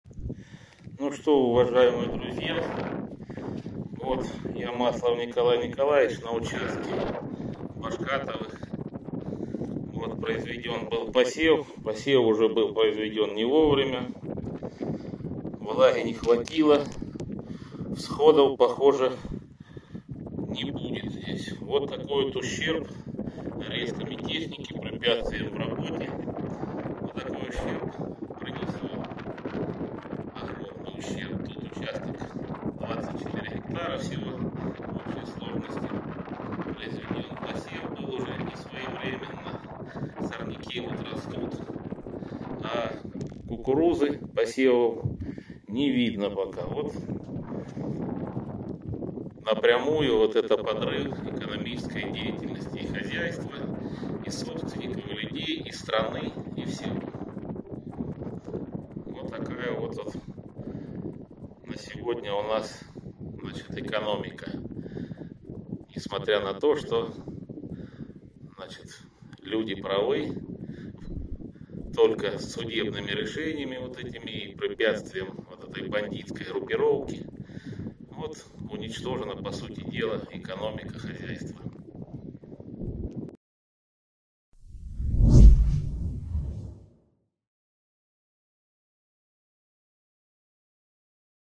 Судебно-бандитская группировка подрывает продовольственную безопасность России — считает фермер
Срочное сообщение пришло с Кубани. Там, в связи с поданными исками, судебные приставы опечатали технику фермера. По словам Николая Маслова ему запрещено использовать даже легковой автомобиль на котором он должен возить в школу детей. Николай Маслов был одним из организаторов тракторного марша и теперь подвергается преследованиям за свою позицию. Он просит оказать ему юридическую и финансовую поддержку. Мы будем признательны всем, кто окажет ему поддержку, а также поможет распространить эту информацию.